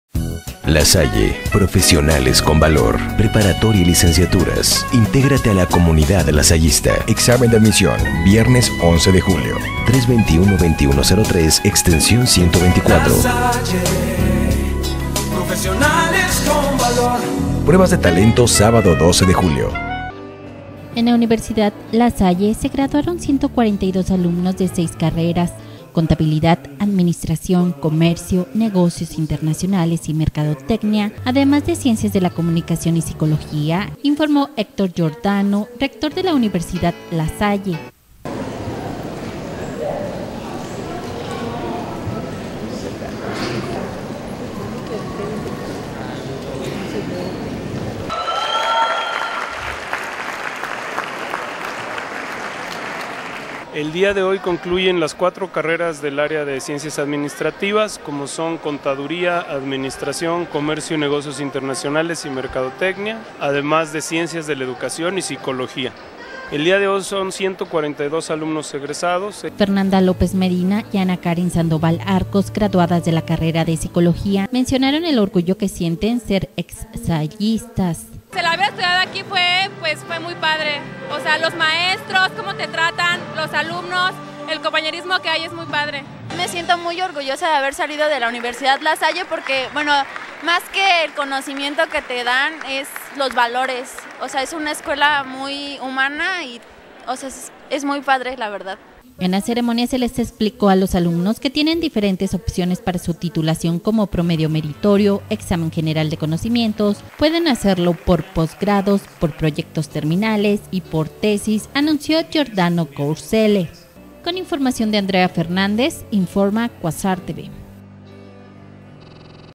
Se gradúan 142 alumnos de La Salle
En La Universidad La Salle se graduaron 142 alumnos de seis carreras: contabilidad, administración, comercio y negocios internacionales y mercadotecnia, además de ciencias de la comunicación y psicología, informó Héctor Giordano Courcelle, rector de la Universidad La Salle...